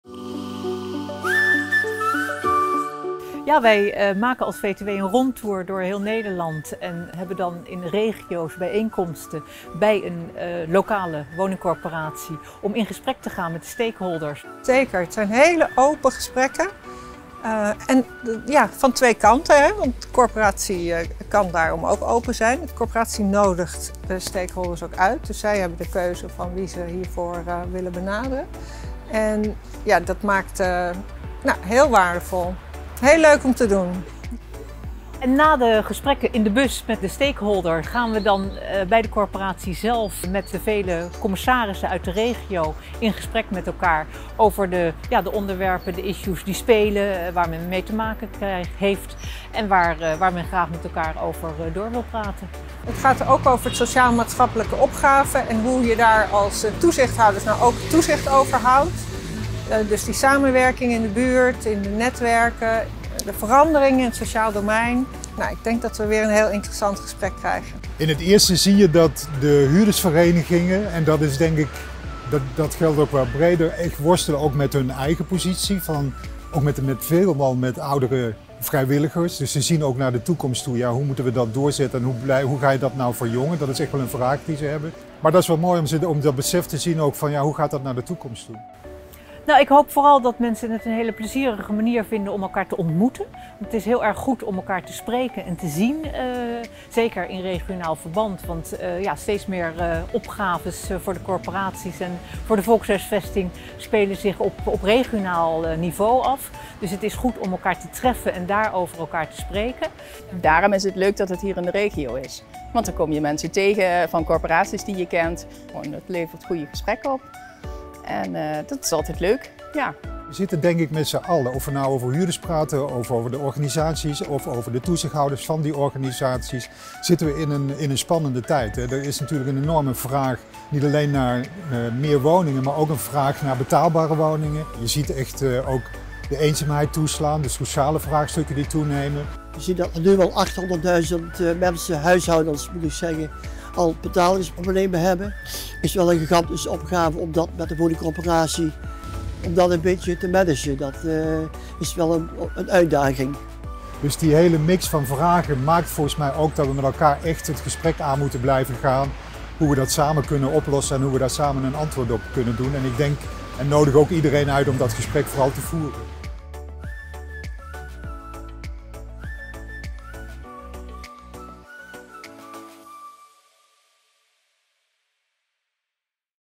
Regionale Bijeenkomsten 2022 ‘De toekomst van het toezicht in de corporatiesector' - Roermond
Op 29 september 2022 was de VTW te gast bij Wonen Limburg in Roermond, voor de zevende en laatste regionale bijeenkomst. In deze video een impressie van de gesprekken met de huurdersvertegenwoordiging en de ledenbijeenkomst 's avonds. Die stond onder meer in het teken van energiearmoede en andere sociale vraagstukken zoals eenzaamheid.